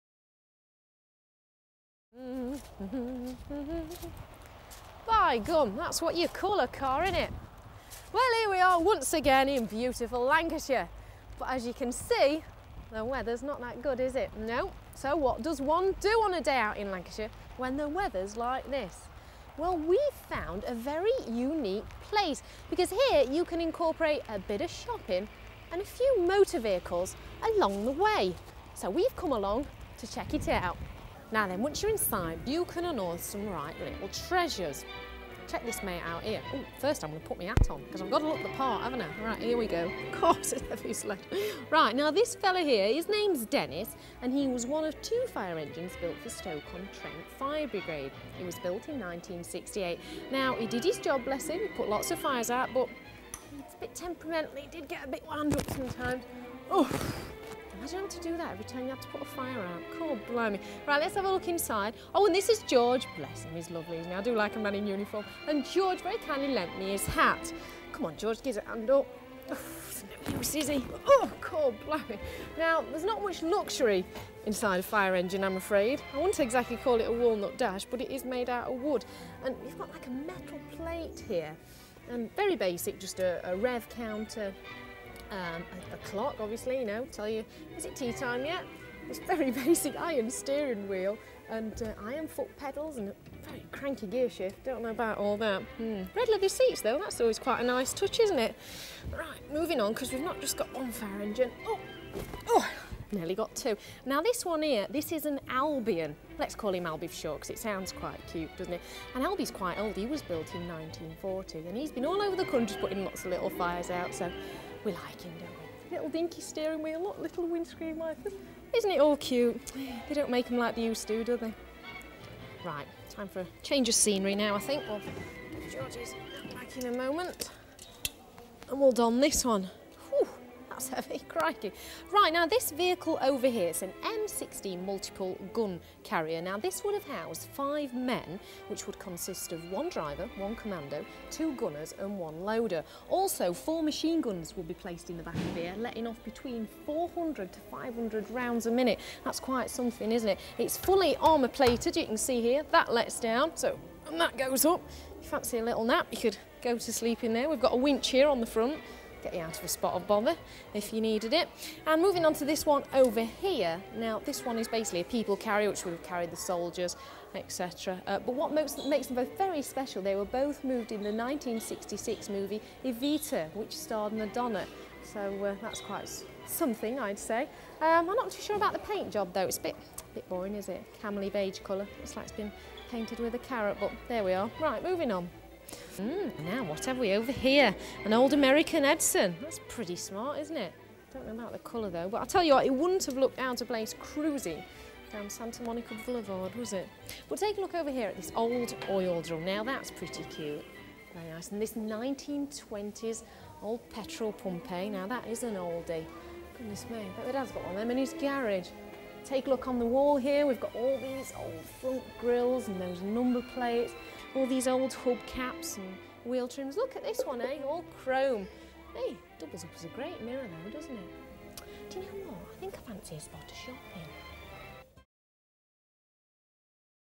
Vintage British Firetruck & American M16 Multiple Gun Carrier
Louise Brady is in Lancashire checking out a few classic motors, one of which is a vintage British firetruck and the other is a ex-military M16 Multiple Gun Carrier. She take a close look inside both vehicles, going through some of their unique features, such as the M16's four machine gun mountings and it's anti-aricraft missiles.